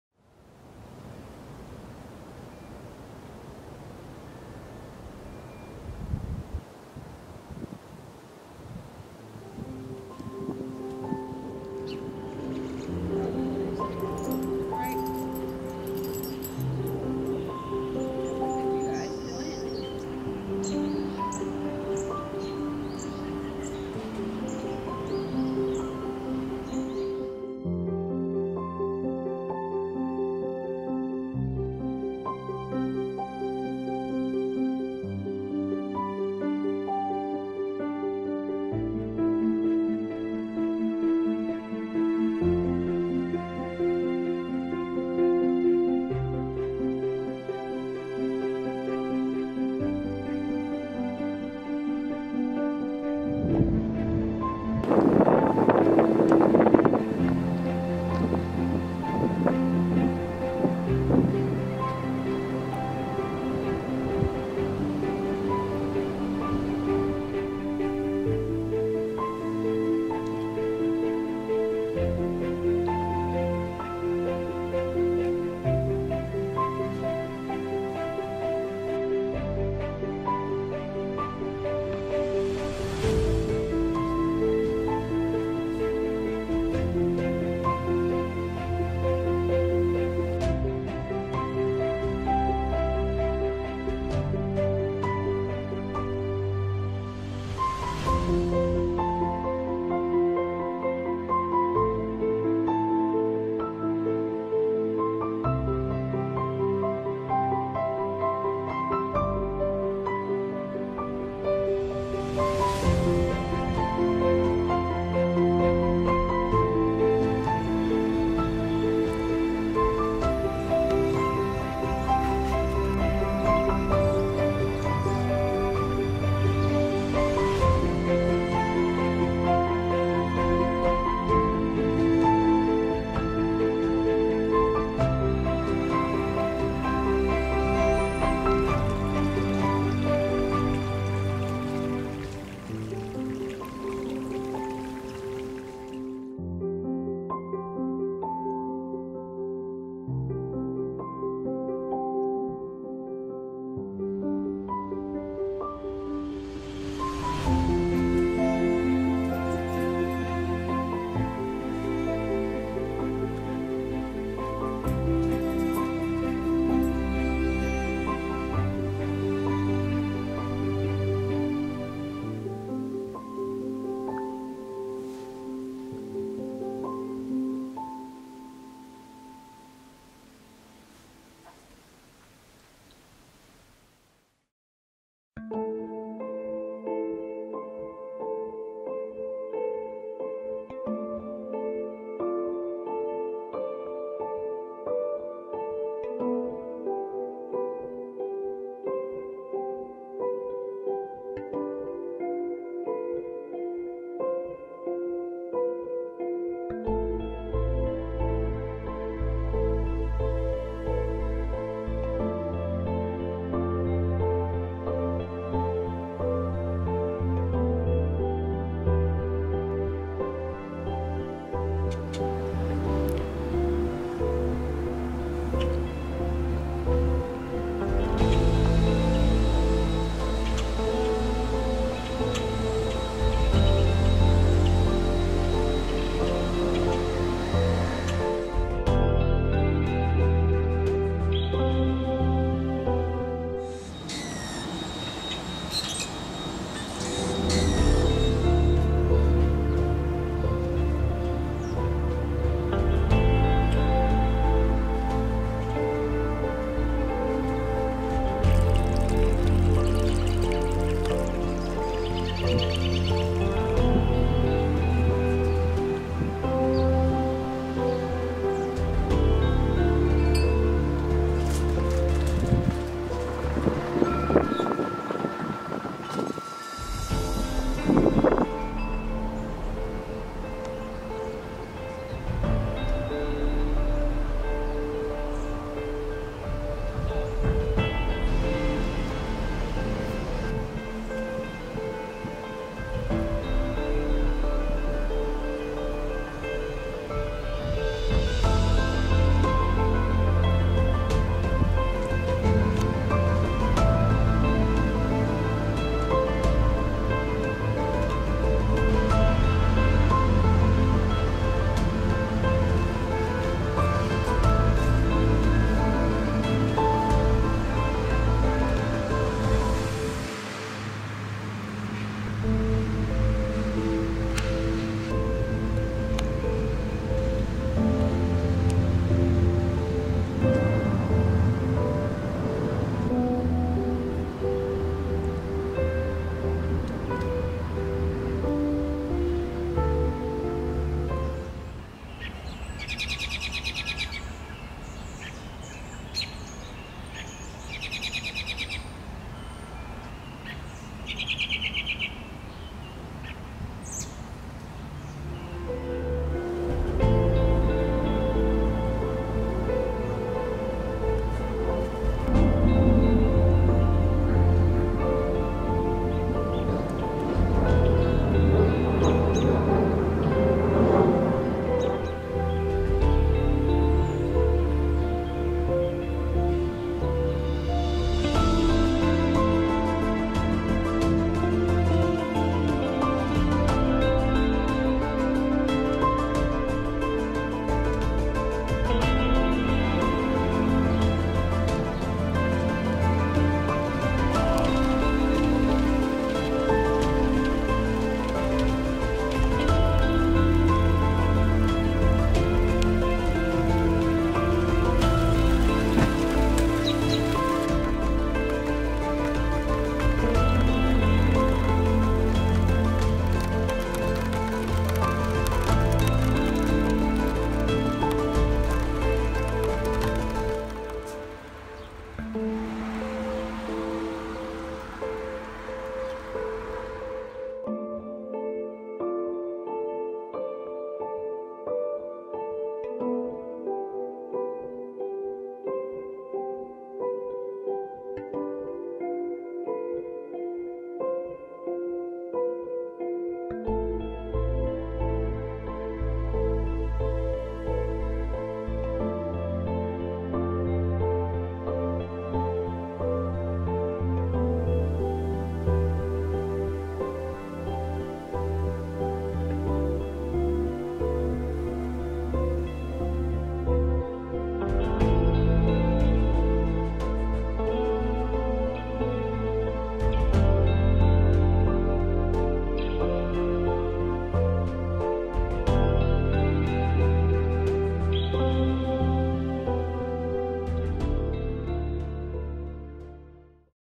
Signs of Spring - Short Film | March-April 2019
A film with footage from late March to early April 2019 in southeast Pennsylvania, including timelapses, footage of birds, and more!